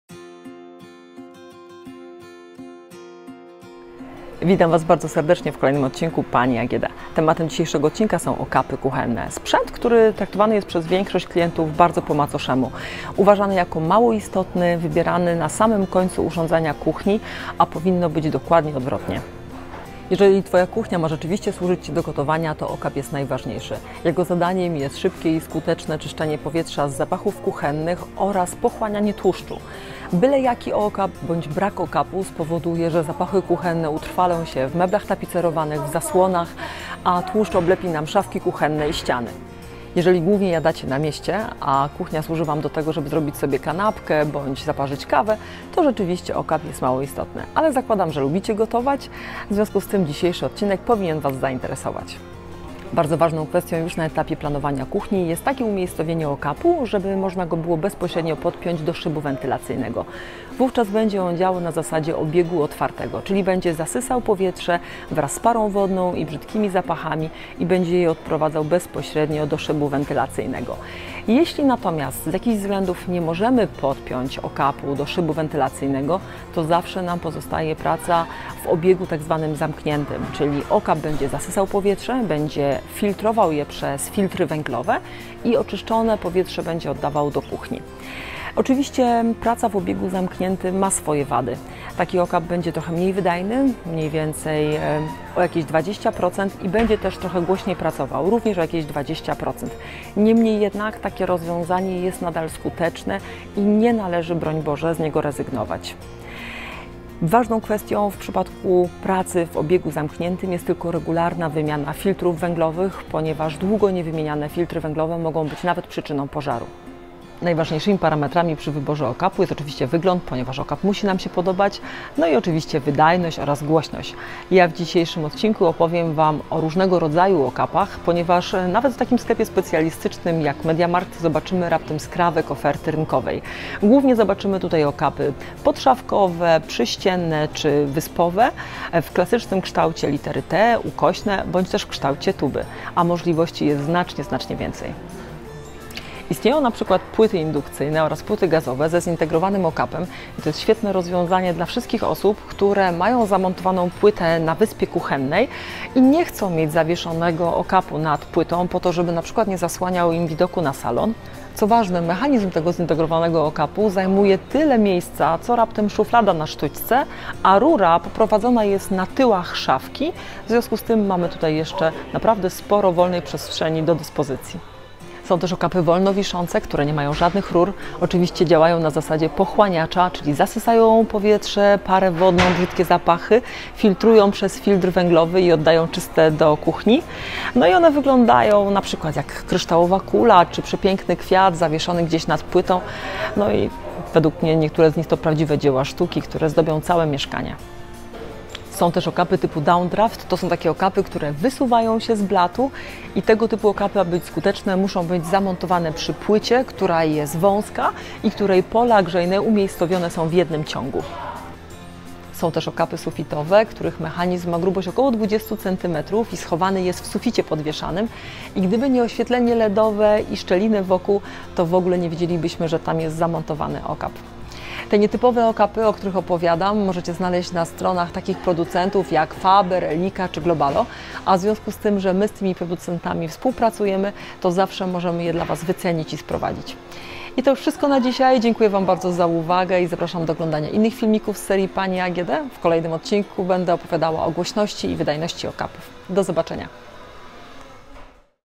Rodzaje okapów kuchennych. Kominowe, podszafkowe, wyspowe, sufitowe i blatowe – który wybrać?
Zadaniem okapu kuchennego jest nie tylko szybkie i skuteczne oczyszczanie powietrza z zapachów, ale również pochłanianie tłuszczu. Okap jest jednym z ważniejszych urządzeń w kuchni, dlatego o wyborze konkretnego rodzaju okapu należy pomyśleć na etapie projektowania pomieszczenia. Zanim podejmiecie jakąkolwiek decyzję najpierw zobaczcie poradnik Pani AGD, która opowiada o  ofercie okapów kominowych, podszafkowych, wyspowych, sufitowych, blatowych, a nawet okapów w formie lampy. Zapraszamy na materiał, który rozwieje Wasze wątpliwości.